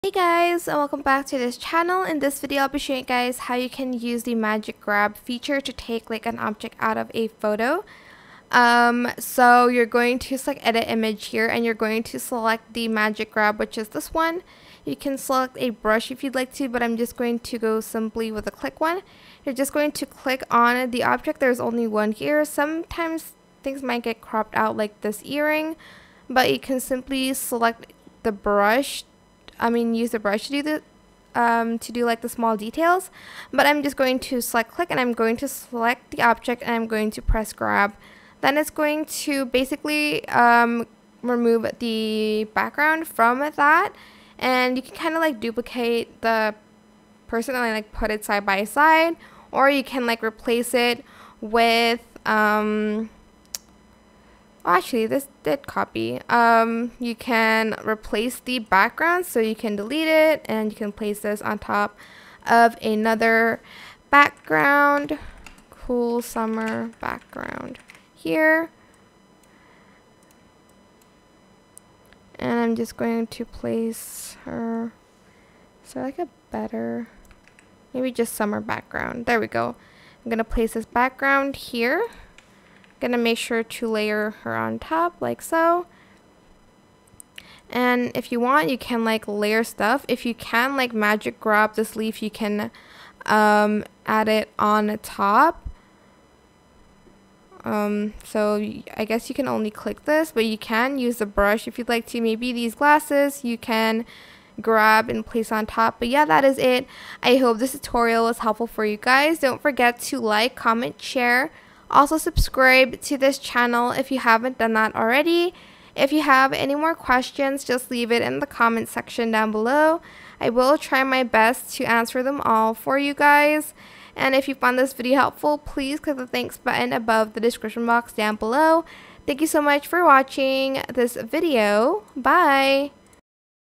How To Use The Magic Grab Feature To Easily Take Out Objects On Photos In Canva
Learn how you can use the magic grab feature in Canva by watching this video.